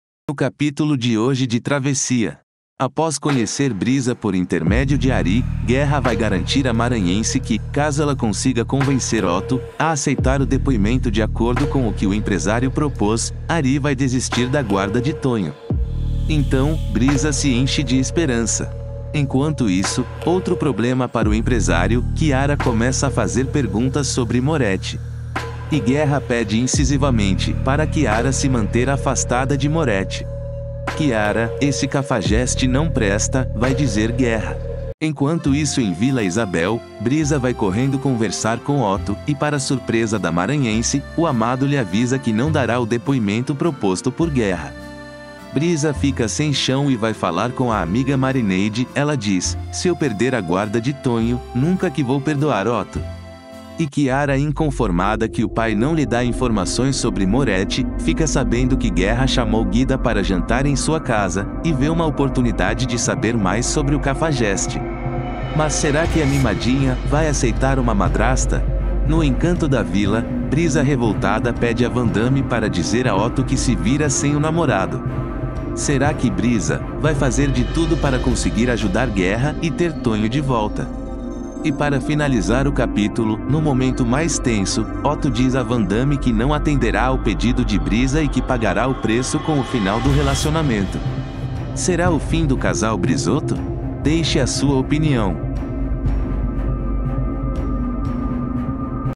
TRAVESSIA - Resumo do 98° Capítulo 31/01 (TERÇA-FEIRA) - Novela Travessia
No capítulo de hoje de Travessia.

Após conhecer Brisa por intermédio de Ari, Guerra vai garantir a maranhense que, caso ela consiga convencer Oto, a aceitar o depoimento de acordo com o que o empresário propôs, Ari vai desistir da guarda de Tonho.

Então, Brisa se enche de esperança.

Enquanto isso, outro problema para o empresário, Chiara começa a fazer perguntas sobre Moretti.

E Guerra pede incisivamente, para Chiara se manter afastada de Moretti.

Chiara, esse cafajeste não presta, vai dizer Guerra.

Enquanto isso em Vila Isabel, Brisa vai correndo conversar com Oto, e para surpresa da maranhense, o amado lhe avisa que não dará o depoimento proposto por Guerra.

Brisa fica sem chão e vai falar com a amiga Marineide, ela diz:

Se eu perder a guarda de Tonho, nunca que vou perdoar Oto.

E Chiara inconformada que o pai não lhe dá informações sobre Moretti, fica sabendo que Guerra chamou Guida para jantar em sua casa, e vê uma oportunidade de saber mais sobre o cafajeste.

Mas será que a mimadinha, vai aceitar uma madrasta?

No encanto da Vila, Brisa revoltada pede a Vandami para dizer a Oto que se vira sem o namorado.

Será que Brisa, vai fazer de tudo para conseguir ajudar Guerra, e ter Tonho de volta.

E para finalizar o capítulo, no momento mais tenso, Oto diz a Vandami que não atenderá ao pedido de Brisa e que pagará o preço com o final do relacionamento.

Será o fim do casal Brisoto? Deixe a sua opinião.